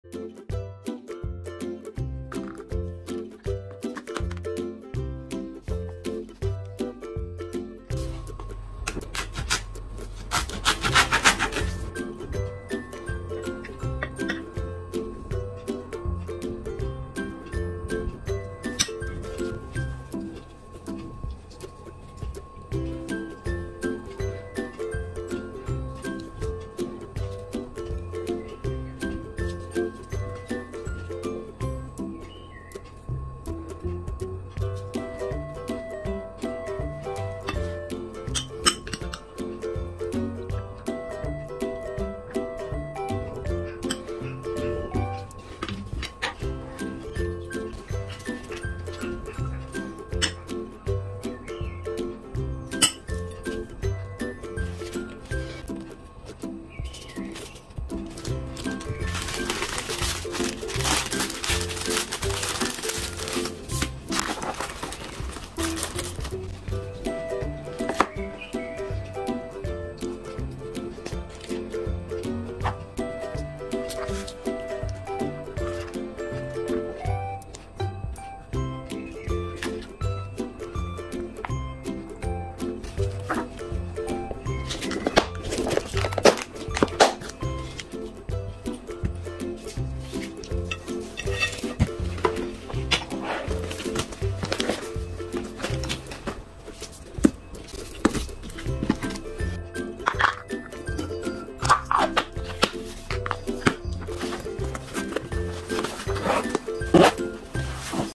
Lunchbox,easy & quick,Coleslaw Roll & More.lunch box recipe
Today's recipe is easy and quick lunchbox with coleslaw roll with with coleslaw roll and more.